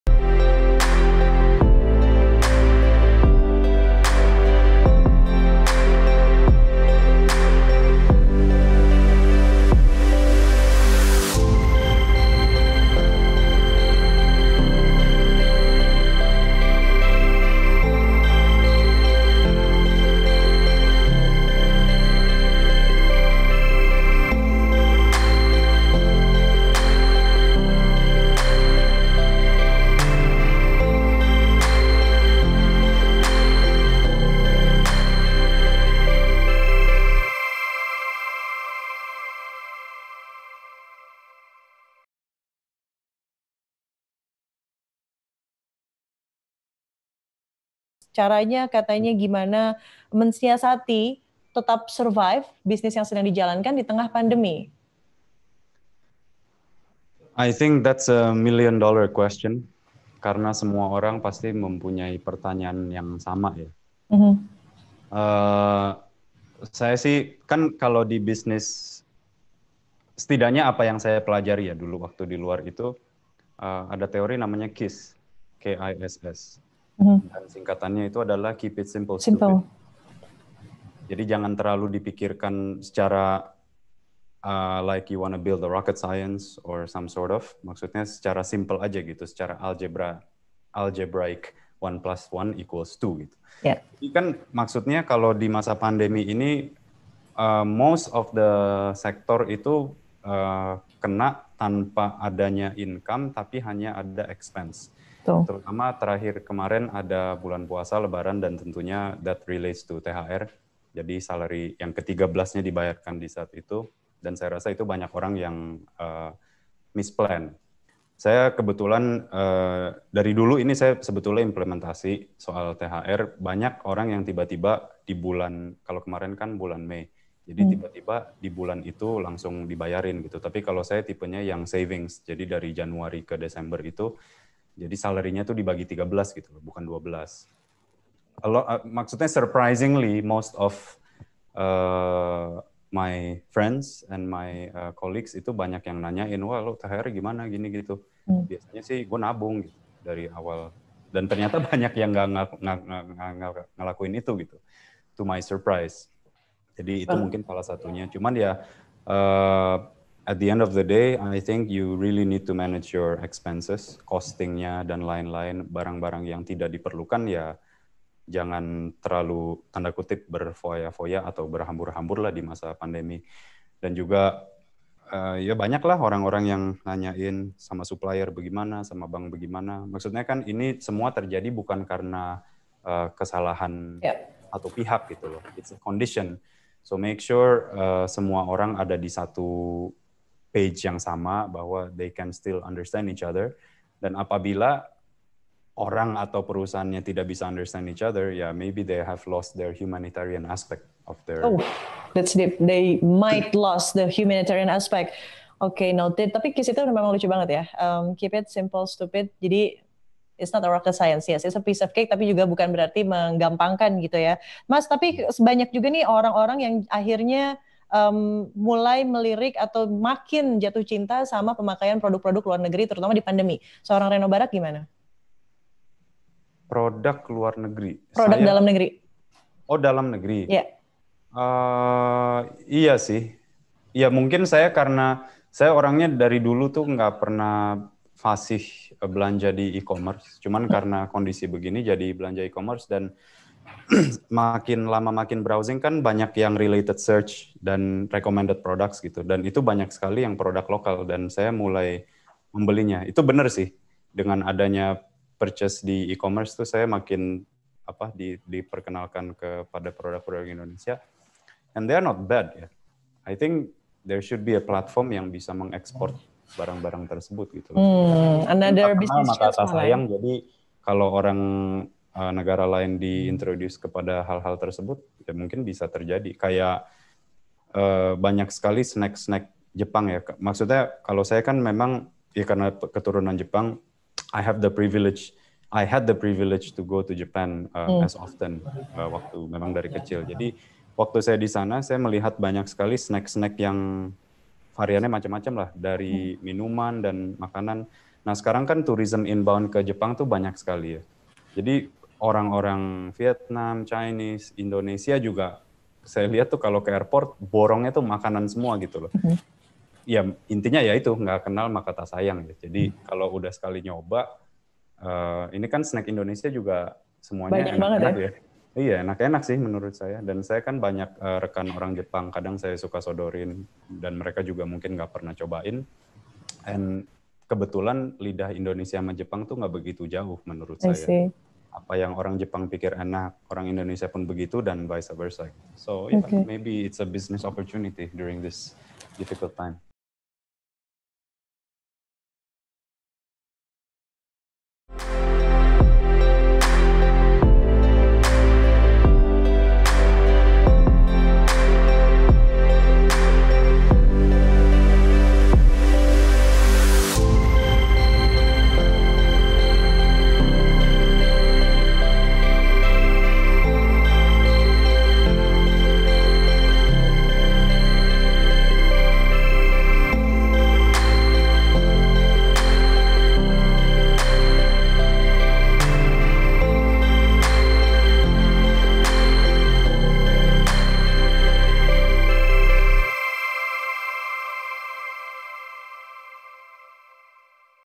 Cara Reino Barack Siasati Bisnisnya di Tengah Pandemi
Nama Reino Barack melesat sebagai salah satu pengusaha muda terkenal di Indonesia. Beragam bidang usaha dia geluti, mulai dari industri media hingga dunia kuliner. Selain pengusaha, suami dari penyanyi Syahrini inijuga dikenal sebagai kreator serial tokusatsu pertama Indonesia, BIMA Satria Garuda.

Lewat program ngobras NgobrolAsyik edisi spesial ditemani host Widya Saputra ini Ia pun berbagi tips atau cara kepada penonton mengenai nasib bisnisnya di tengah pandemi. Kita ketahui pandemi COVID-19 ini memaksa kita semua untuk bisa bertahan dari segala macam cobaan, salah satunya ekonomi. Selama pandemi banyak masyarakat Indonesia yang terdampak, misalnya terkena pemutusan hubungan kerja (PHK). Dengan begitu penghasilan setiap bulannya pun terganggu. Para pelaku bisnis pun mencari cara bagaimana usaha nya bisa tetap bertahan.

Reino Barack pun mempunyai cara tersendiri agar bisninya tetap bertahan. Ia menggunakan teori KISS (Keep It Simple Stupid). Apa maksudnya? Penasaran bagaimana strategi Reino Barack? Keep watching this video.